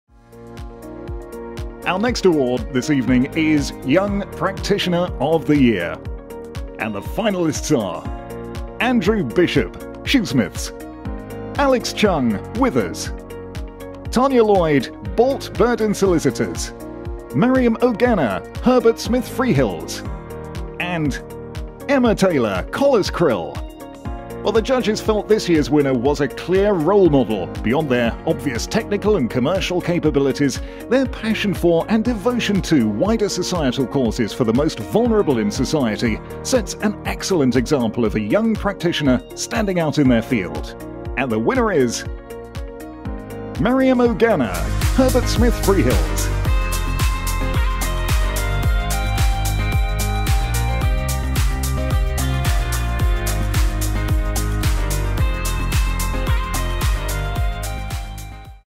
STEP PCA 2020: Young Practitioner of the Year
Maryam Oghanna of Herbert Smith Freehills receives the 2020 STEP Private Client Award for Young Practitioner of the Year